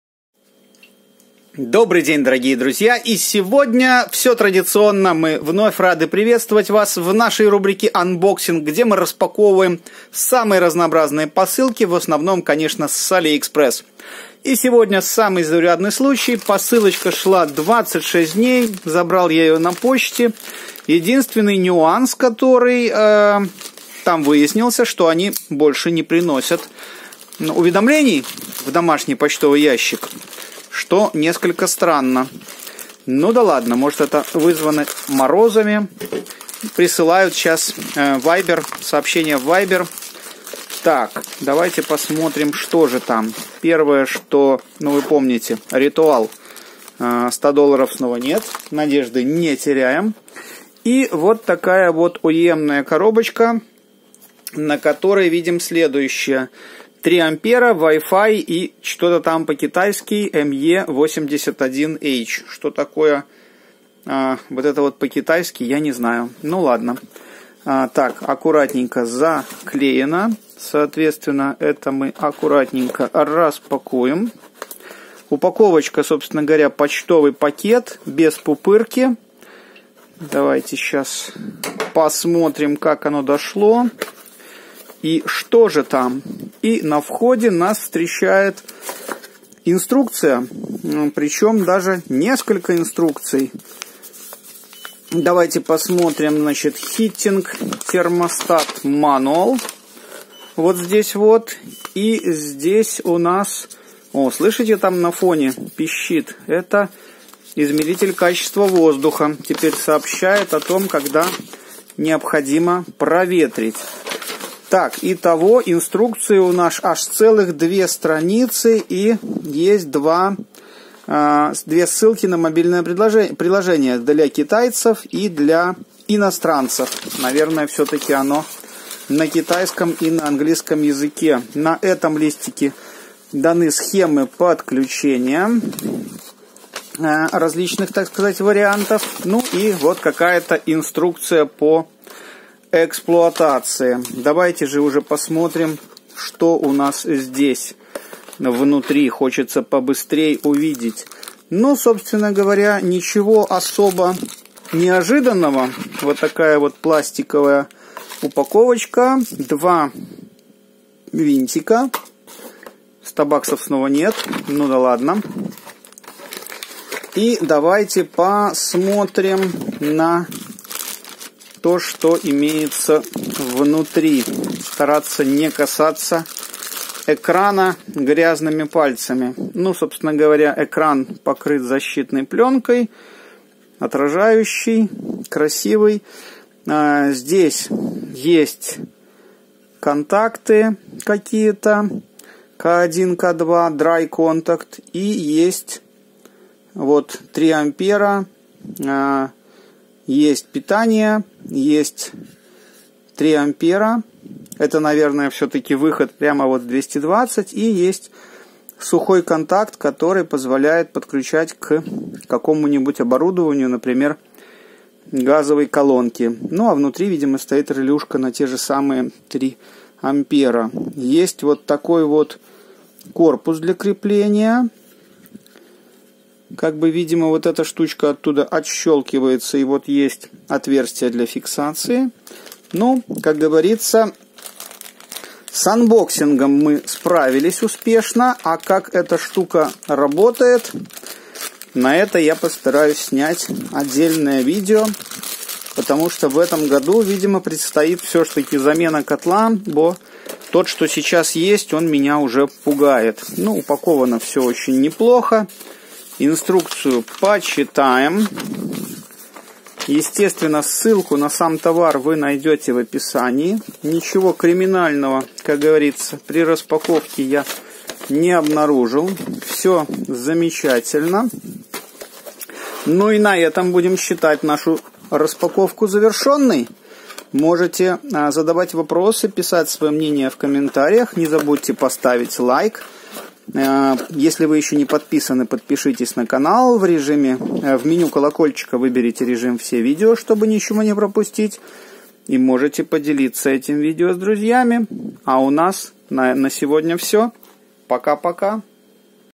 Unboxing:AVATTO Tuya WiFi Smart Thermostat, Electric Floor Heating Water/Gas Boiler Temperature
Unboxing:AVATTO Tuya WiFi Smart Thermostat, Electric Floor Heating Water/Gas Boiler Temperature Remote Controller for Google Home, Alexa
I'm very satisfied. Отправка 1 день. Доставка 26 дней. Трек валидный. Упаковка достаточная. Товар соотвествует описанию. Товар и продавца рекомендую.  В работе еще не проверял.